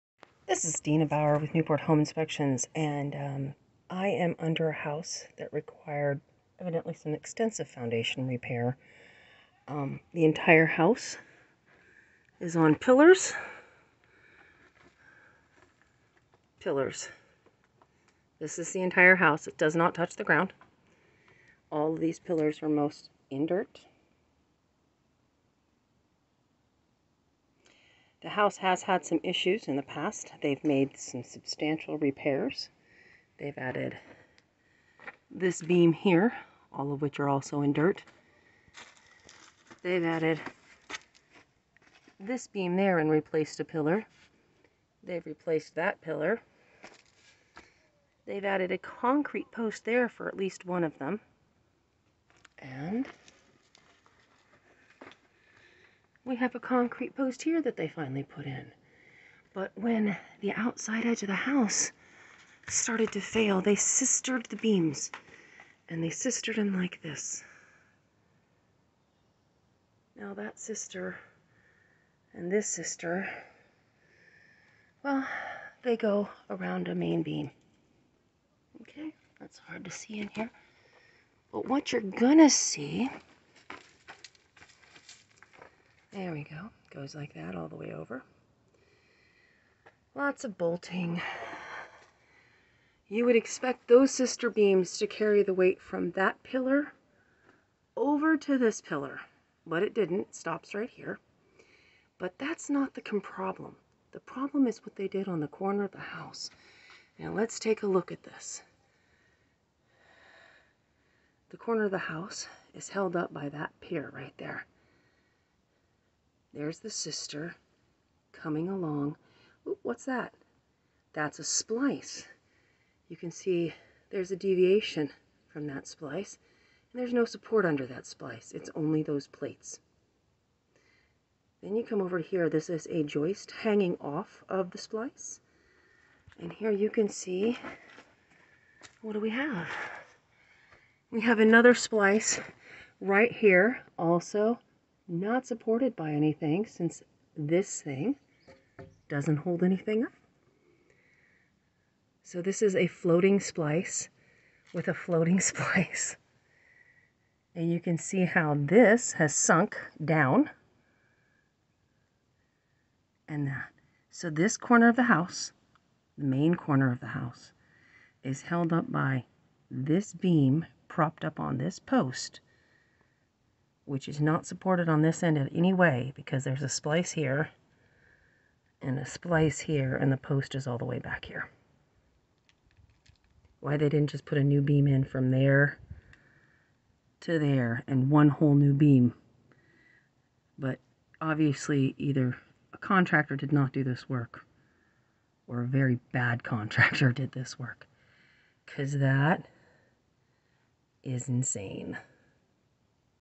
Foundation Repair Failed! Worst spliced beam I have ever seen.
BAD foundation repairs. Insane. can't believe a contractor did this!!!